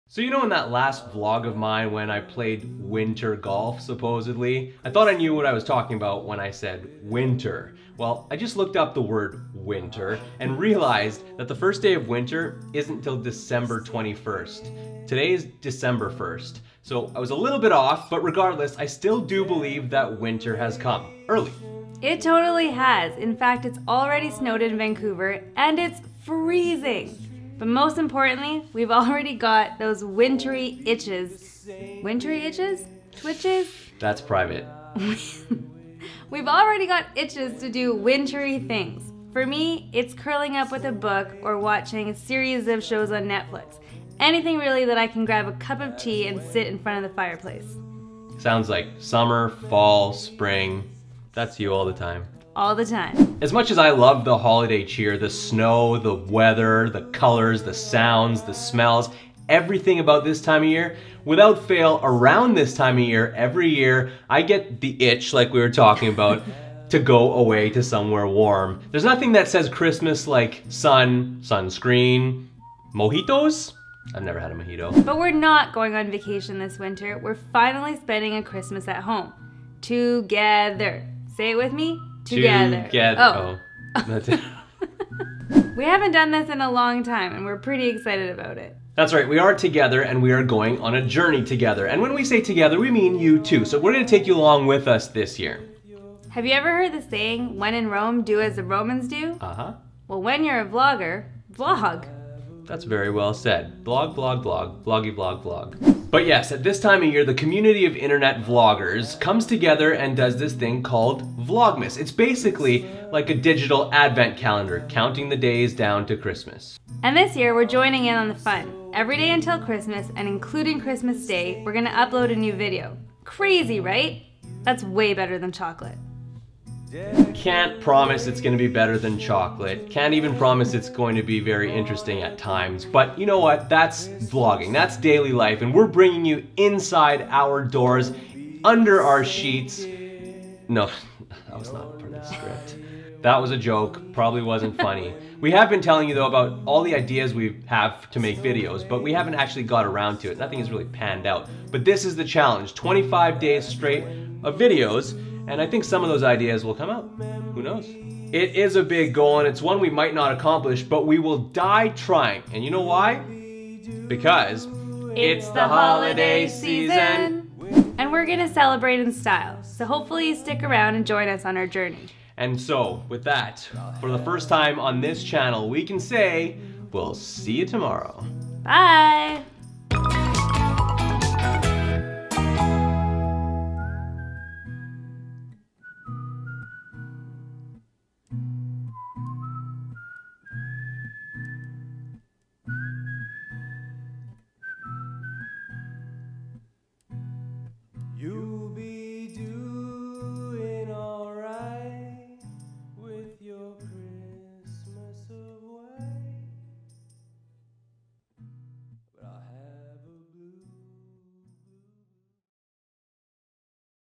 Vlogmas: Our Digital Advent Calendar
We are celebrating Vlogmas 2014 by video blogging every day from December 1st to December 25th! Our first video shares our plans.